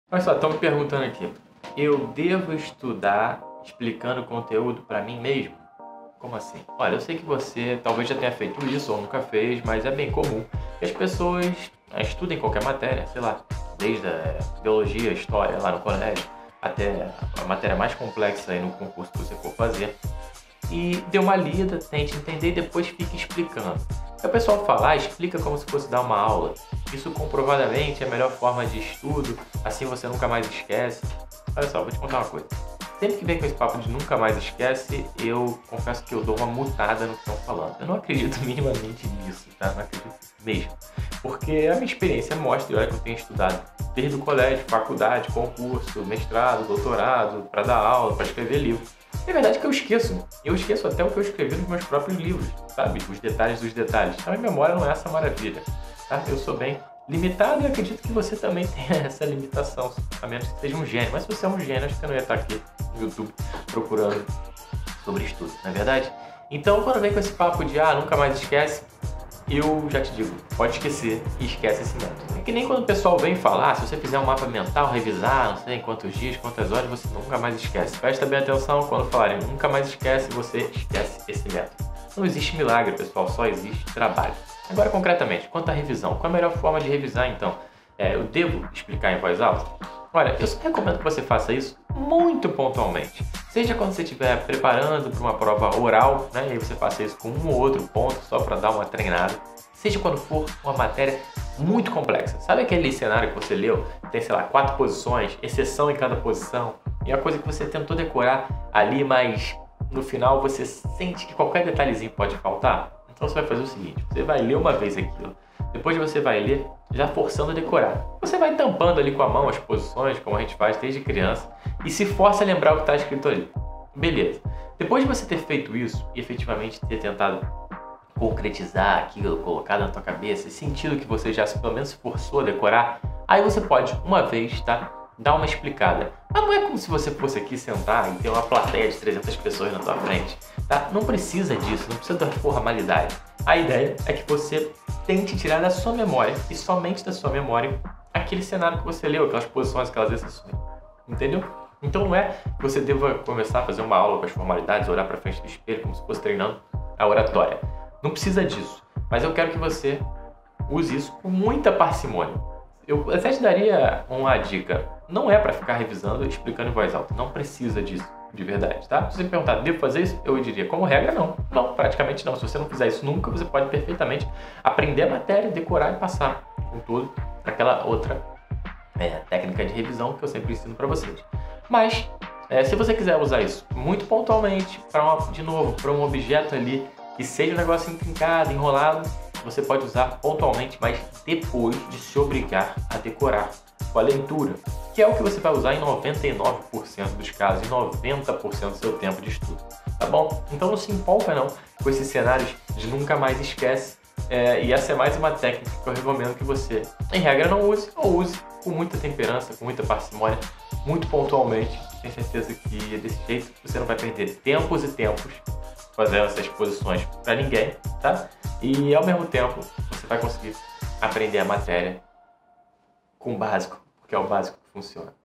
Para decorar as matérias, eu preciso explicar para mim mesmo?
Aprenda de uma vez por todas se você deve ou não repetir para você mesmo a matéria para fixá-la na sua cabeça.

Entre na lista de espera do Até a Aprovação, o meu programa de treinamento que te ensina a passar mais rápido em concursos públicos